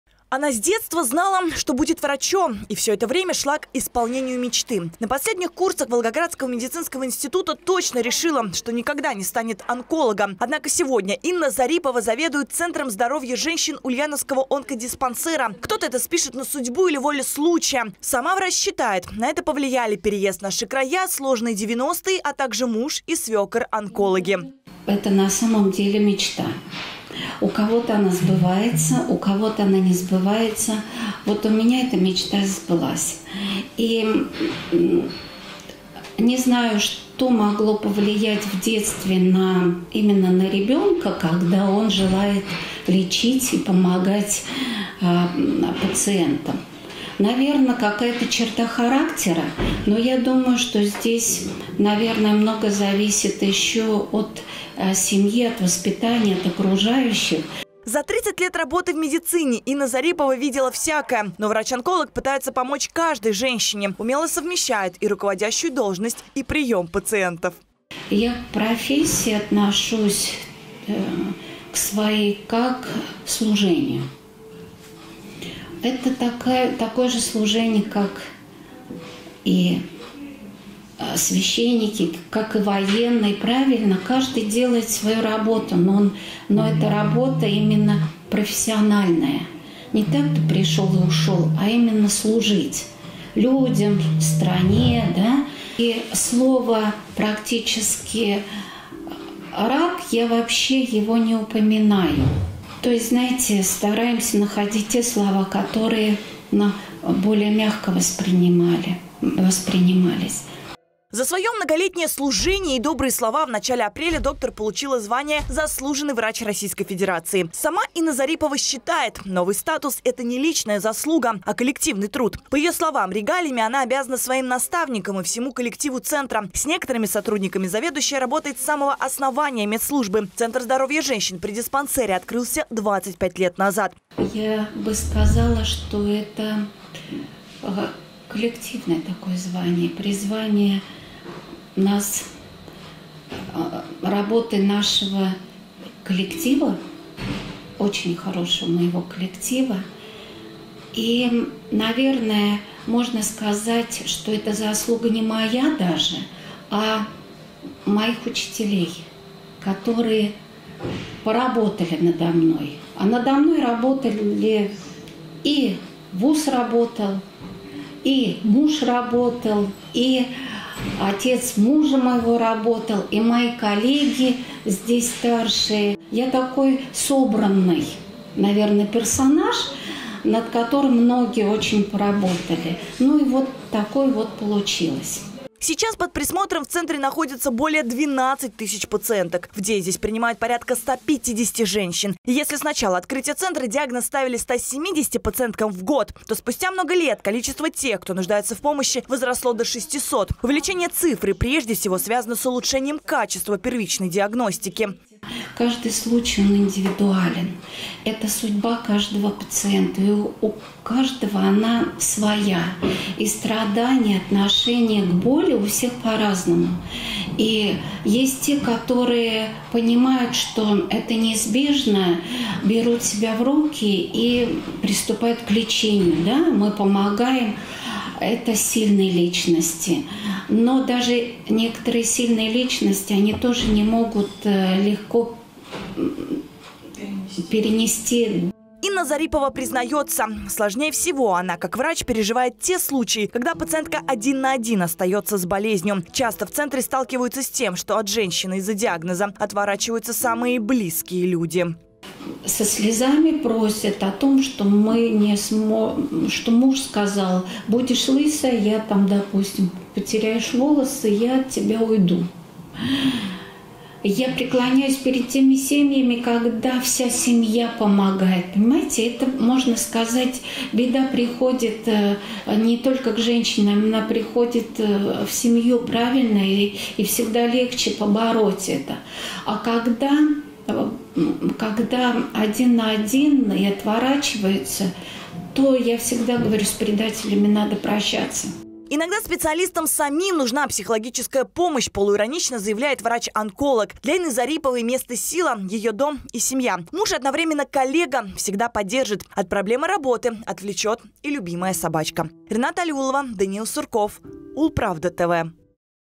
Врач-онколог Инна Зарипова: «Я – собранный персонаж»
В начале апреля двум ульяновским медикам присвоили звания «Заслуженный врач Российской Федерации». Соответствующий указ подписал глава государства Владимир Путин. Среди лучших -  заведующая Центром здоровья женщин Ульяновского областного клинического онкологического диспансера Инна Зарипова. О том, как  жизнь привела доктора в  отделение онкологии, и с чем до сих пор не может смериться врач - в нашем сюжете.